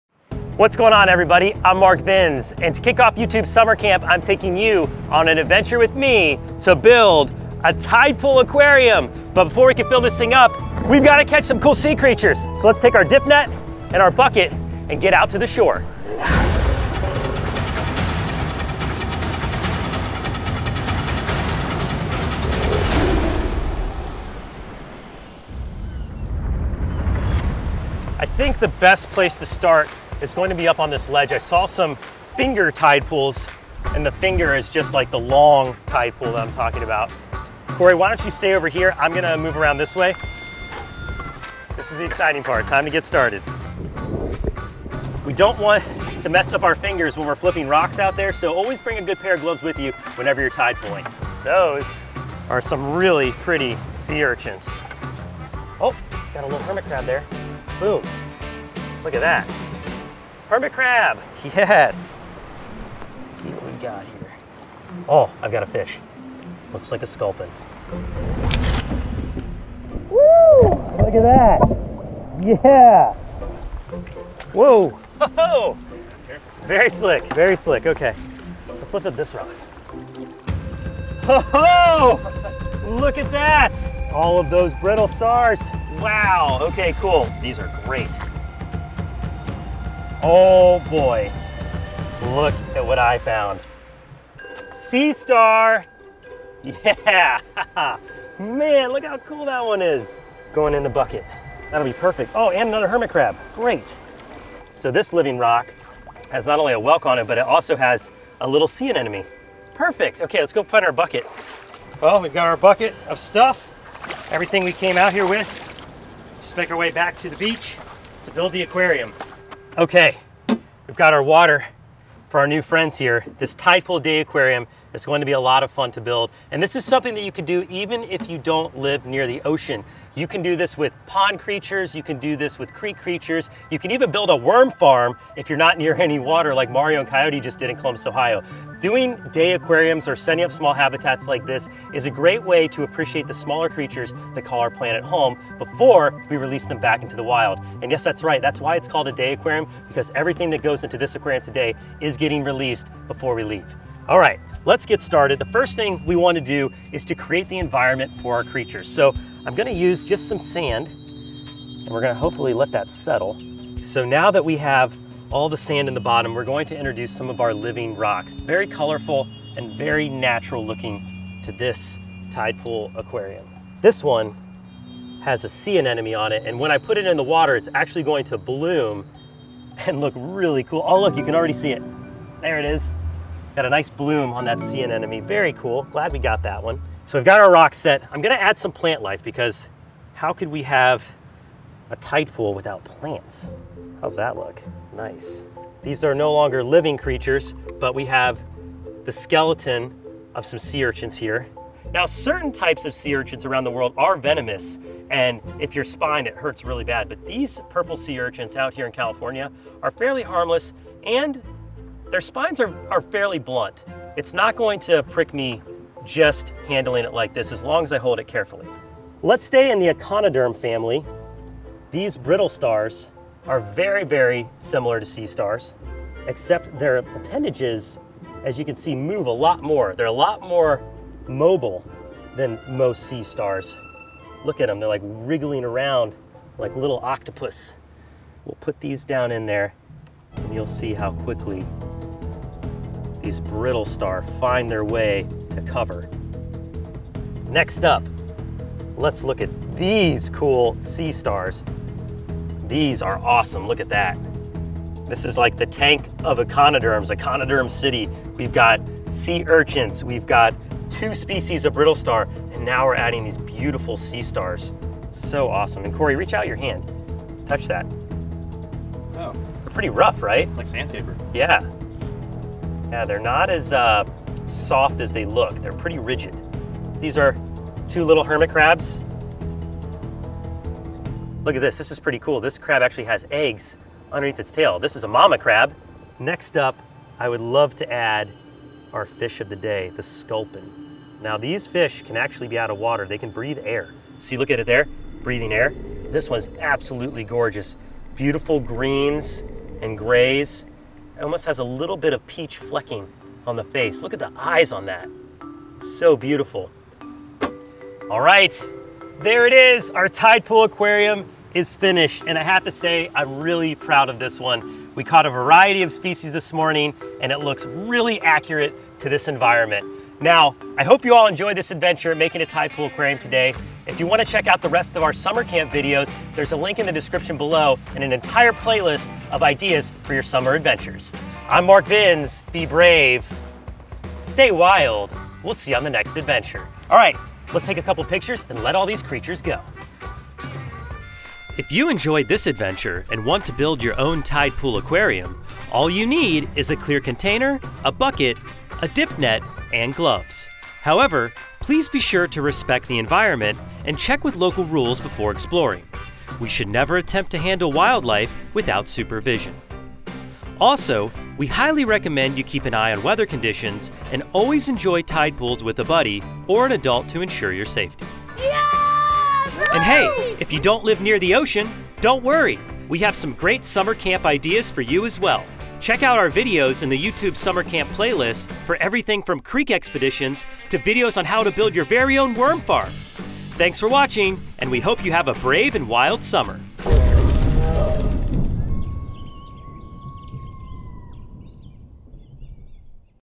Make a Tide Pool Aquarium for CampYouTube!
On this episode, Mark takes you tide pooling for CampYouTube! Watch as Mark shows you how to build a tide pool aquarium full of brittlestars, sea stars, and more! Get ready to meet some super cool Tide Pool creatures!

The Brave Wilderness Channel is your one stop connection to a wild world of adventure and amazing up close animal encounters.